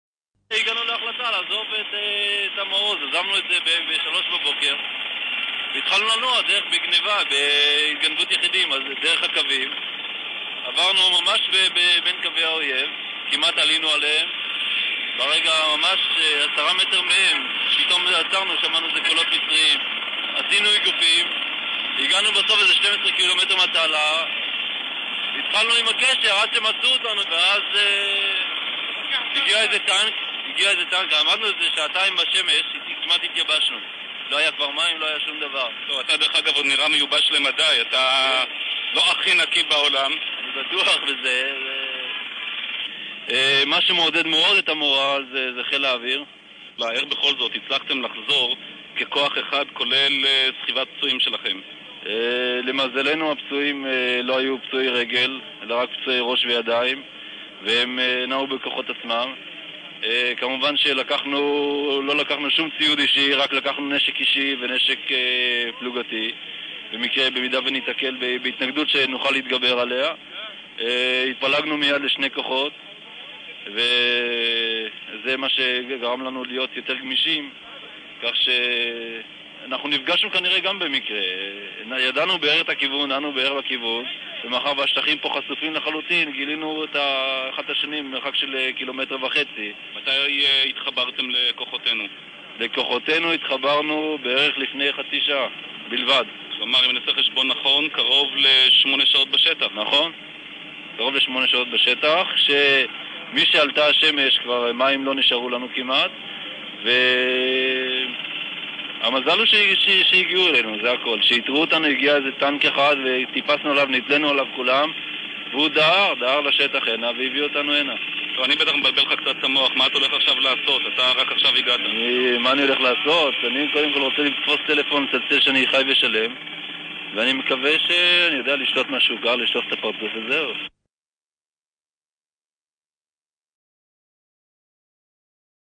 מלחמת יום הכיפורים 948 1
מיכה שגריר - ראיון קטוע שערך במערב סיני בזמן קרבות הבלימה בחזית המצרית עם חייל/קצין מילואים ממעוז "פורקן", ממעוזי "קו בר-לב", שמספר על ההיחלצות של אנשי המעוז (ההקלטה מתחילה בדברי החייל). החייל מספר שבשלוש לפנות בוקר הם החליטו לנטוש את המעוז והתחילו לנוע דרך קווי המצרים, כאשר במהלך התנועה הם שמעו קולות של מצריים ועשו איגוף עד שהגיעו לנקודה במרחק 12 ק"מ מתעלת סואץ, ושם חיכו יותר משעה תחת השמש הקופחת עד שהגיע טנק מחלץ וכמעט התייבשו. שגריר מעיר שהוא אכן נראה מיובש לגמרי ושואל איך הם הצליחו לחזור ככוח אחד כולל סחיבת הפצועים. החייל משיב שלמזלם כל הפצועים היון פצועים בראש או בידיים ולא ברגליים, וכך הפצועים נעו בכוחות עצמם. הוא מוסיף שהם התפצלו לשני כוחות וזה איפשר להם להיות יותר גמישים, ובהמשך הם מצאו אחד את השני. אחרי שמונה שעות שהיו בשטח הגיע טנק החילוץ, כל אנשי המעוז טיפסו עליו, והוא לקח אותם לשטח שבשליטת צה"ל.
(CD  ,4)

0010sri